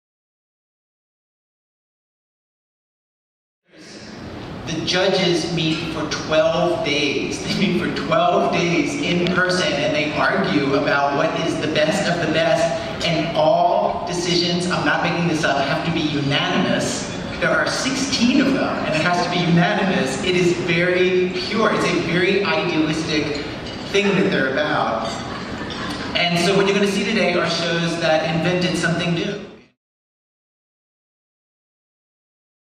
Ira Glass speaks about prestige of Peabody Awards
Ira Glass of "This American Life" hosted the 2014 Peabody Awards on May 19, 2014.